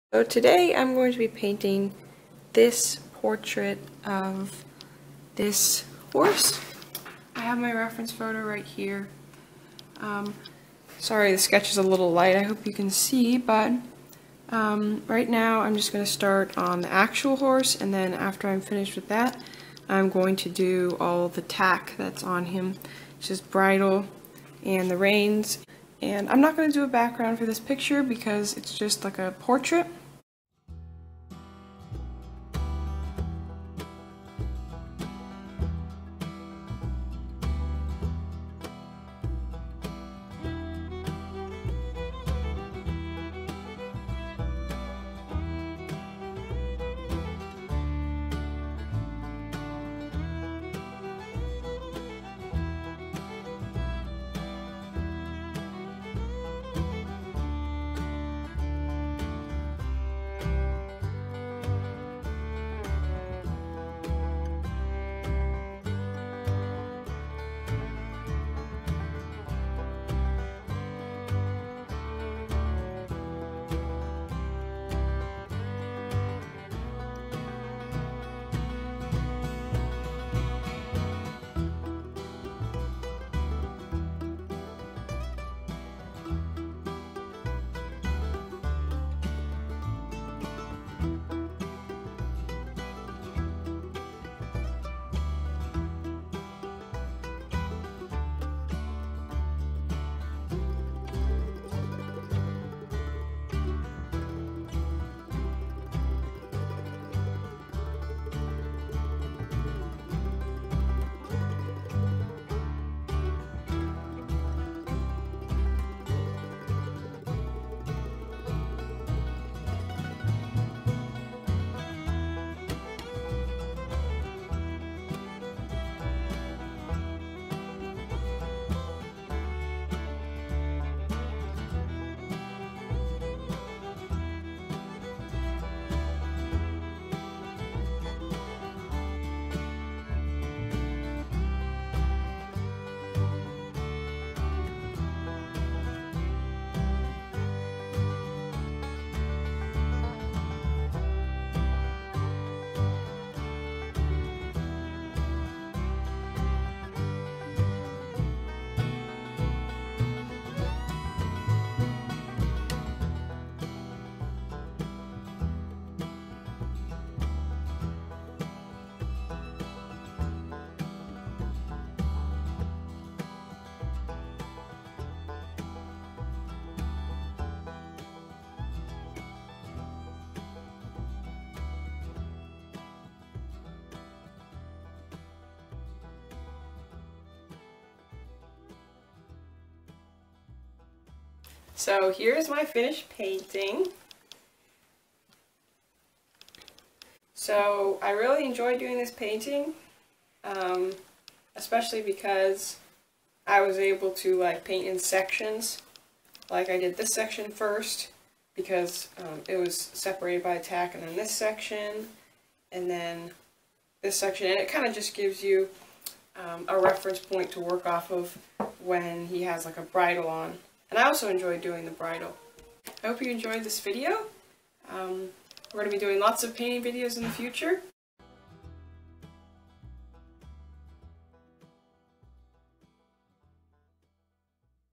Horse Wearing Bridle Watercolor Portrait
In this video, E paints a nice watercolor horse wearing a bridle.

Links:
Etsy Shop